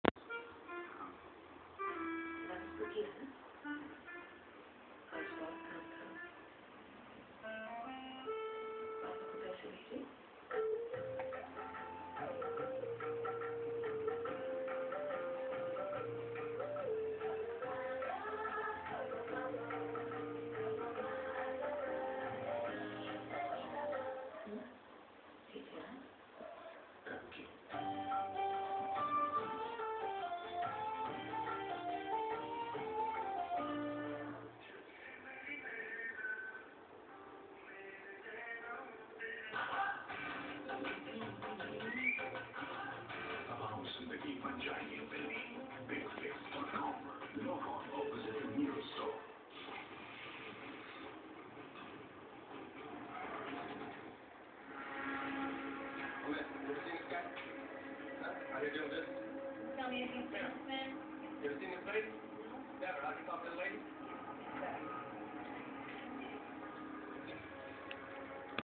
Arrow - Avi n Steffi's dog
cute small pomenerian dog. we juss love him